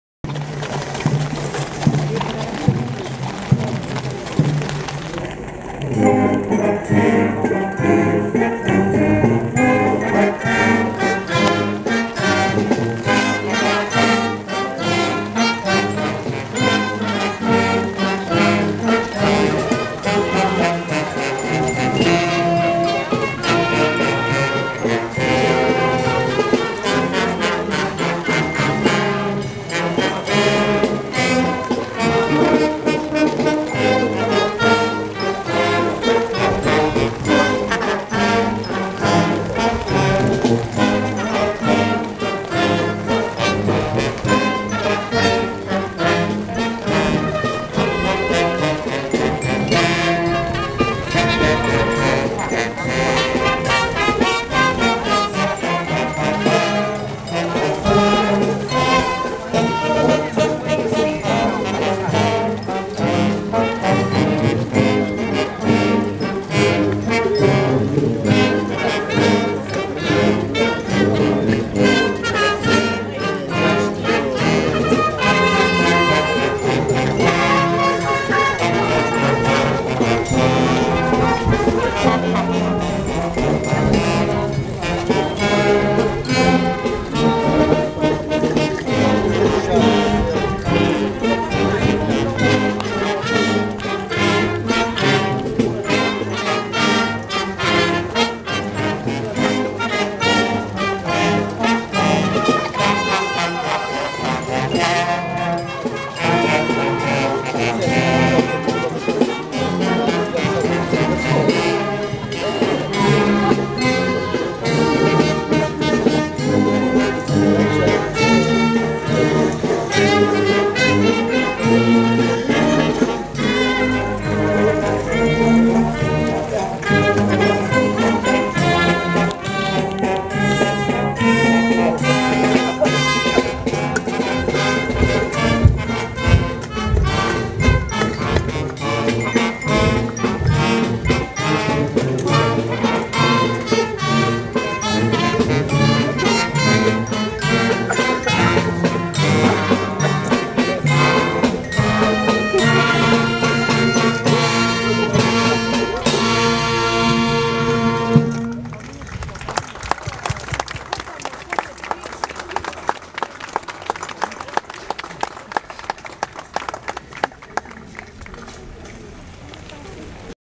Charanga a cavalo da GNR -- Pedrógão Grande 2018- video Joao viola - 5
Pedrógão Grande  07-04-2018 - Jardim da  Deveza.
Comemorações do dia do Comando Territorial de Leiria da GNR em Pedrógão Grande. 7 e 8 de Abril
nem a chuva perturbou o espetaculo.video joao viola